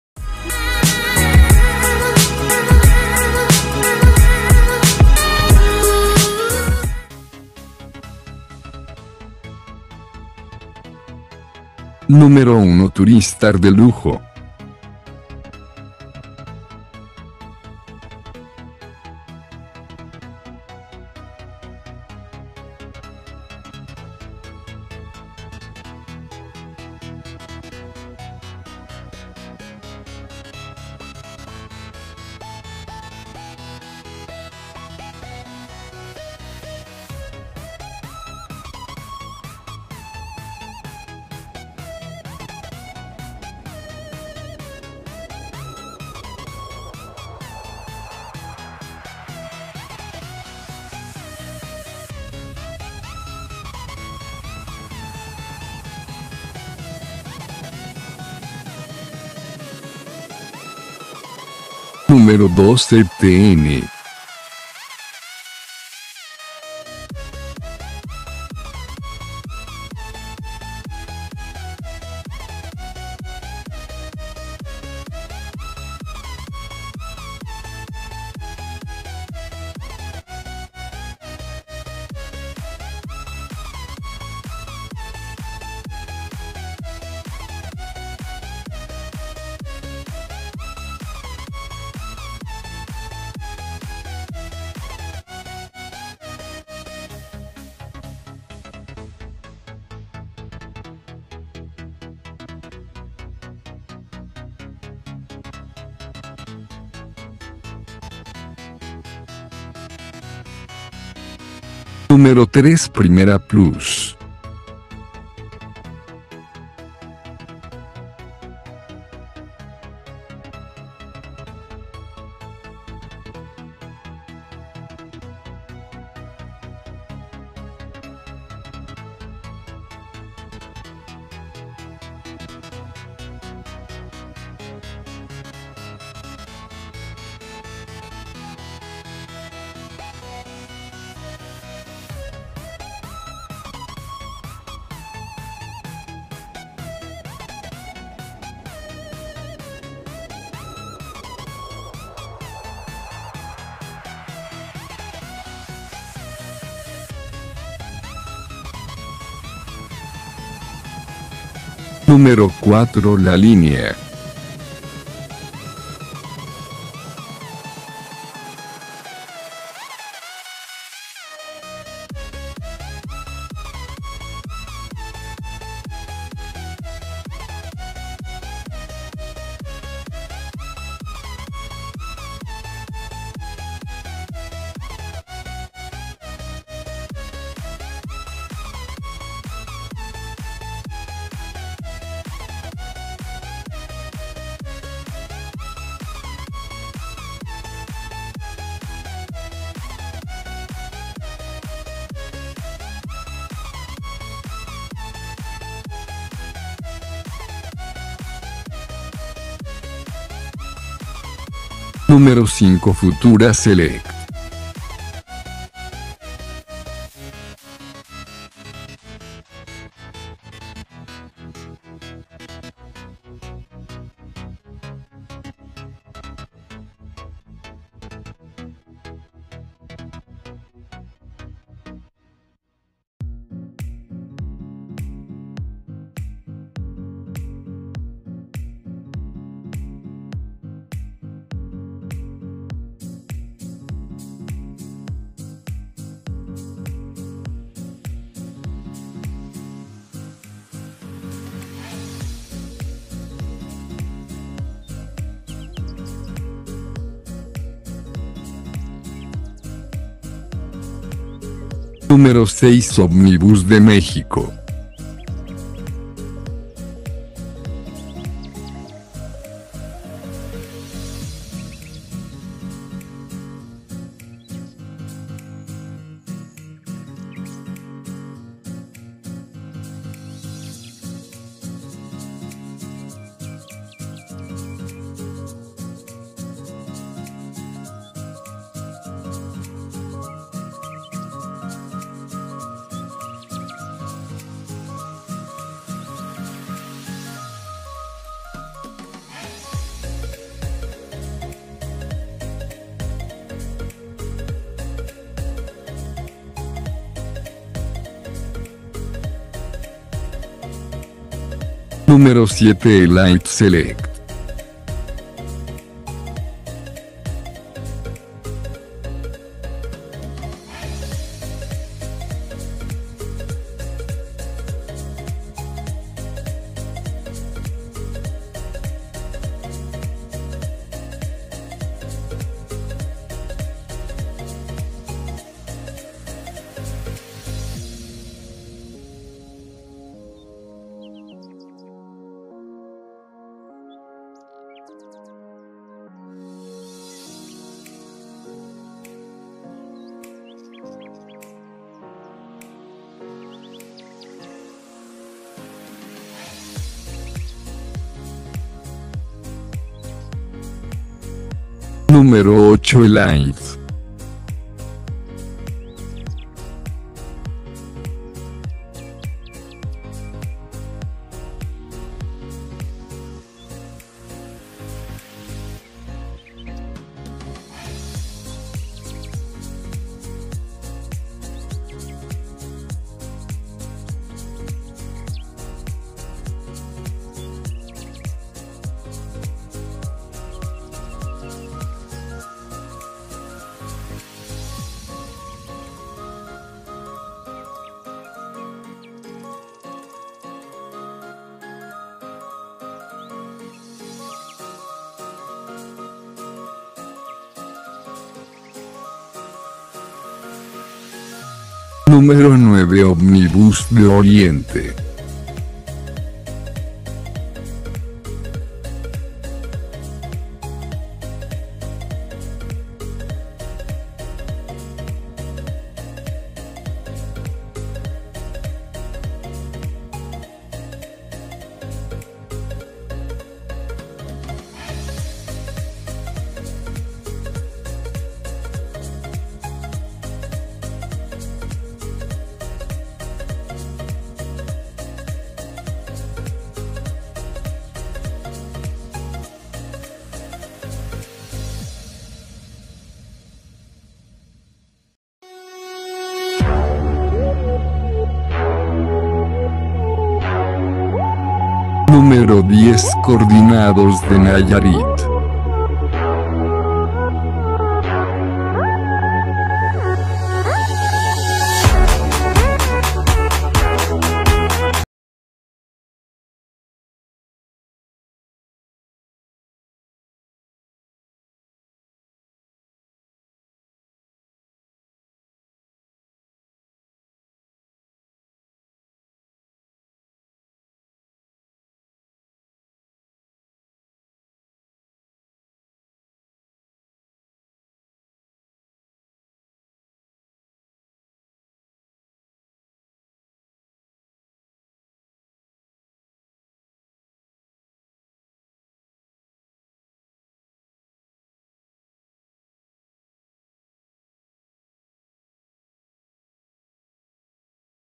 Top 10|Líneas de Autobuses que viajan a Guadalajara
Amigos les dejo este video de 10 líneas que viajan a Guadalajara,Jalisco
Espero y les agrade y no olviden
Dejar su like
Gracias :)